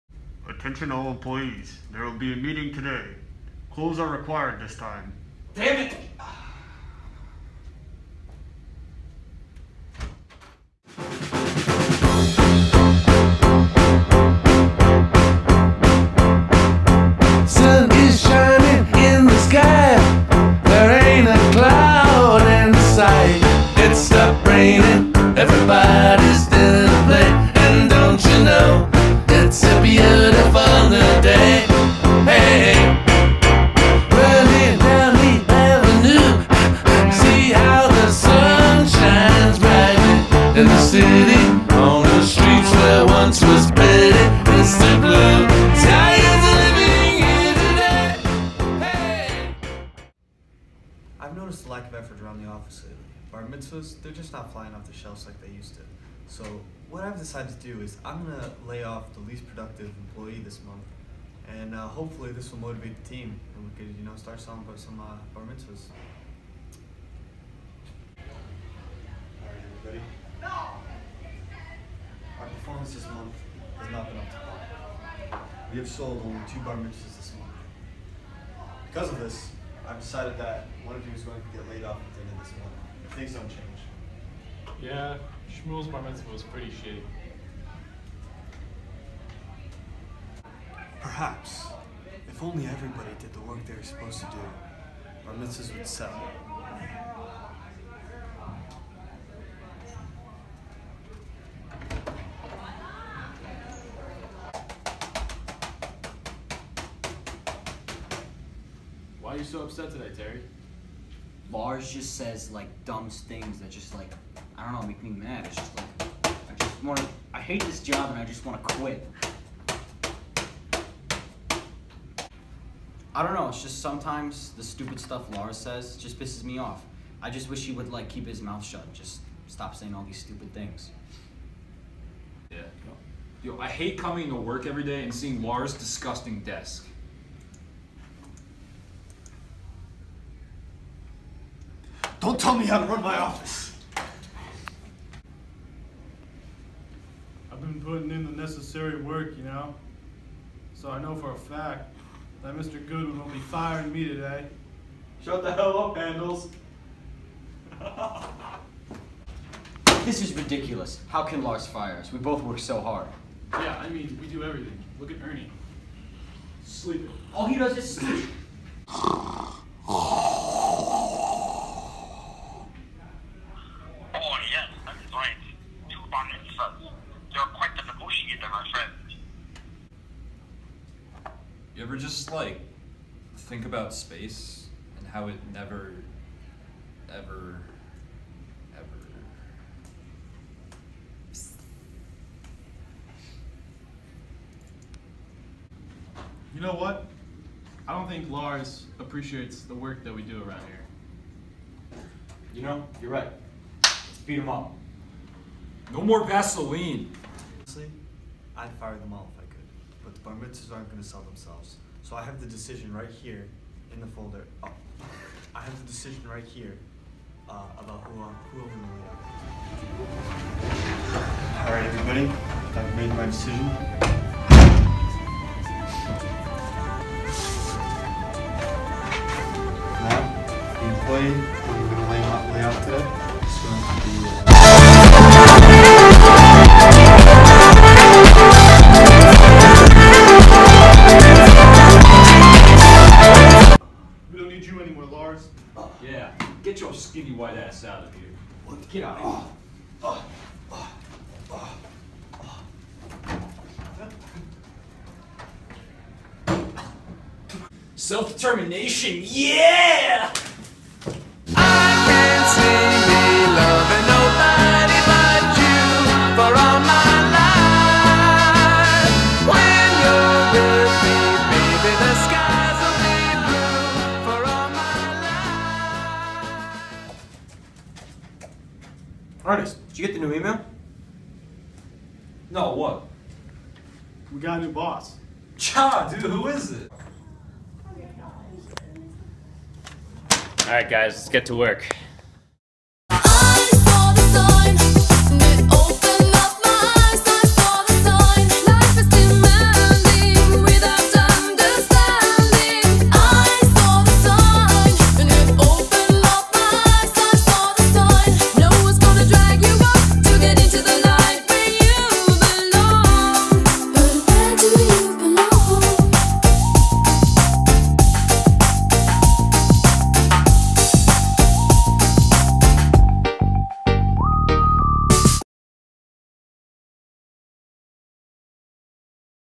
The Judd Road Office
Prepare to laugh, cry, and love with this years new hit show, The Judd Road Office! Watch these lovable characters face crazy shenanigans as they go through the day working at their local Bar Mitzvah dealership.

Fun for the whole family unit!